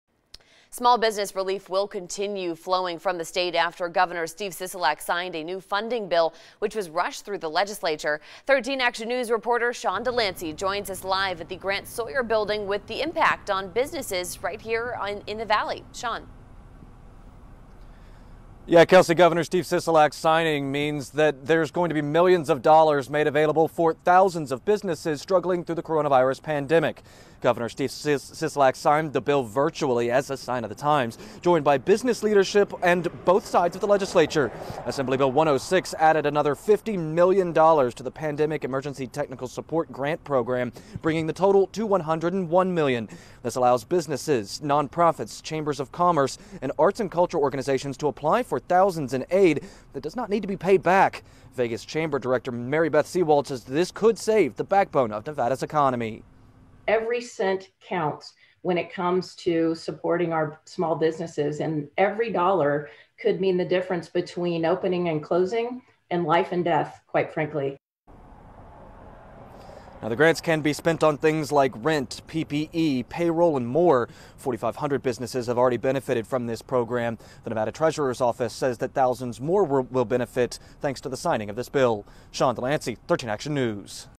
New small business aid bill signed by Nevada governor
Governor Steve Sisolak signed a bill appropriating $50,000,000 more dollars to small businesses affected by the coronavirus pandemic.